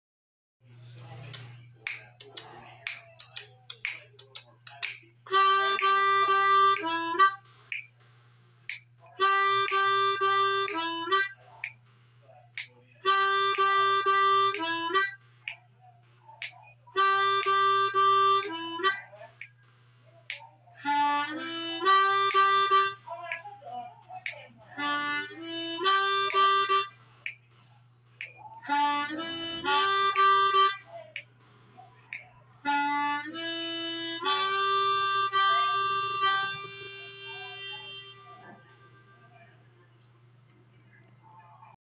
Just Two Licks (Blues Harmonica Tutorial #2) From @TomlinHarmonica Tomlin Leckie
I wanna thank you for your feedback from my last tutorial from Juzzie Smith. But this time, I learned two licks from Tomlin Leckie from @TomlinHarmonica Please let me know if you like my tutorial! Thank you Tomlin! I will keep doing my best.